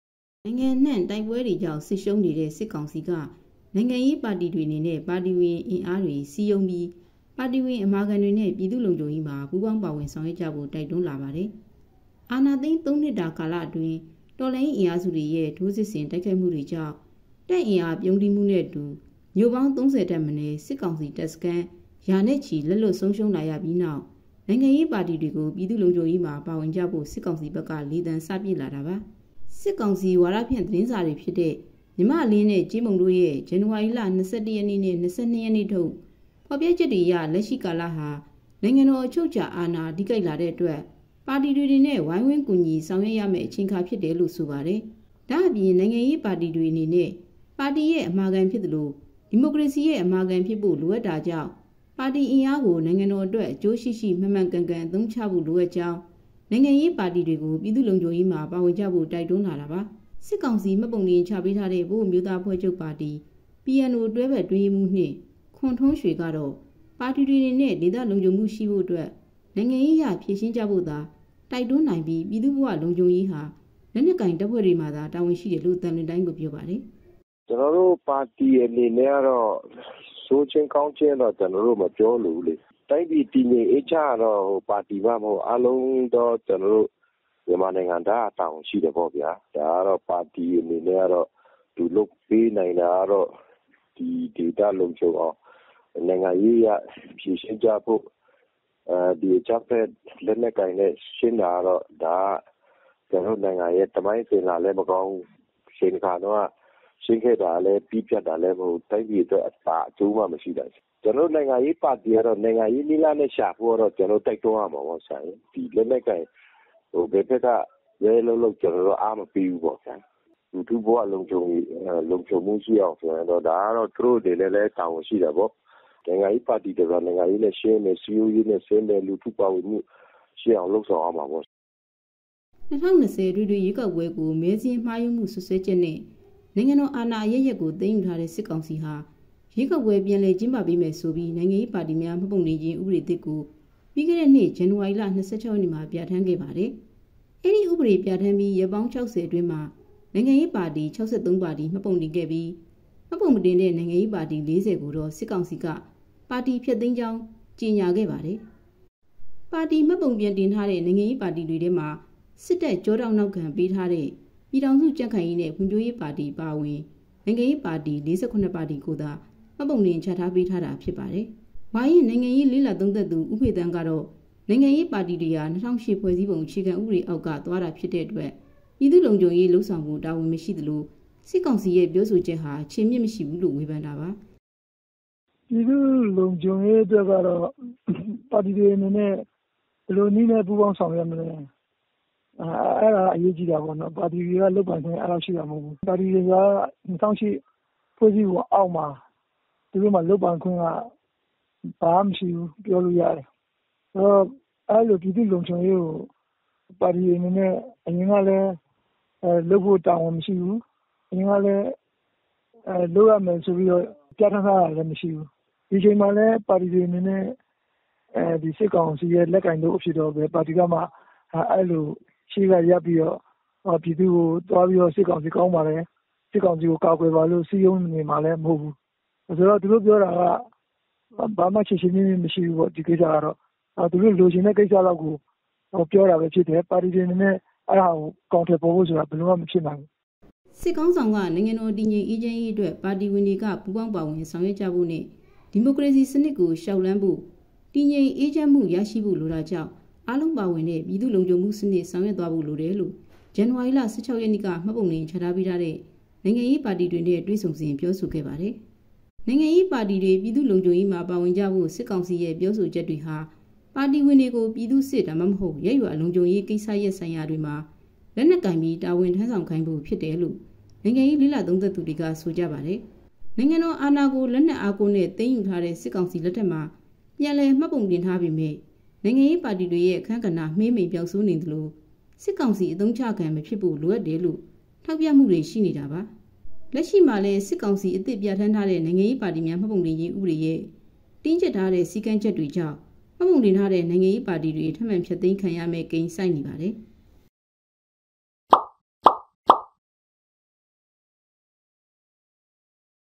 TLT Tv||- နိုင်ငံရေးပါတီတွေ ပြည်သူ့လုံခြုံရေးမှာ ပါဝင်ကြဖို့ စစ်ကောင်စီ လေသံစပစ်
နိုင်ငံအနှံ့ တိုက်ပွဲတွေကြောင့် စစ်ရှုံးနေတဲ့ စစ်ကောင်စီက နိုင်ငံရေးပါတီတွေအနေနဲ့ ပါတီဝင်အင်အားတွေ စည်းရုံးပြီး ပါတီဝင်အမာခံတွေနဲ့ ပြည်သူ့လုံခြုံရေးမှာ ပူးပေါင်းပါဝင်ဆောင်ရွက်ကြဖို့ တိုက်တွန်းလာပါတယ်။

အာဏာသိမ်း သုံးနှစ်တာကာလအတွင်း တော်လှန်ရေးအင်အားစုတွေရဲ့ ထိုးစစ်ဆင်တိုက်ခိုက်တွေကြောင့် တပ်အင်အားပြုန်းတီးမှုနဲ့အတူ မြို့ပေါင်း ၃၀ ထက်မနည်း၊ စစ်ကောင်စီတပ်စခန်း ရာနဲ့ချီ လက်လွှတ်ဆုံးရှုံးလာရပြီးနောက် နိုင်ငံရေးပါတီတွေကို ပြည်သူ့လုံခြုံရေးမှာ ပါဝင်ကြဖို့ စစ်ကောင်စီဘက်က လေသံစပစ်လာတာပါ။